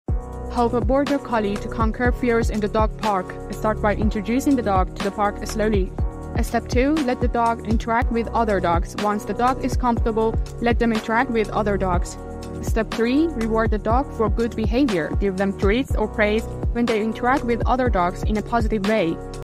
Border Collie conquers fears in the dog park
Dog training is an important part of owning a pet, and it can be especially important for a Border Collie. Border Collies are known for their intelligence and energy, and they can be easily overwhelmed in a dog park. With the right training, however, a Border Collie can conquer their fears and become a confident and happy pup in the dog park. With patience and consistency, a Border Collie can learn to interact with other dogs, play fetch, and even learn tricks. With the right training, a Border Collie can become a well-rounded and confident pup in the dog park.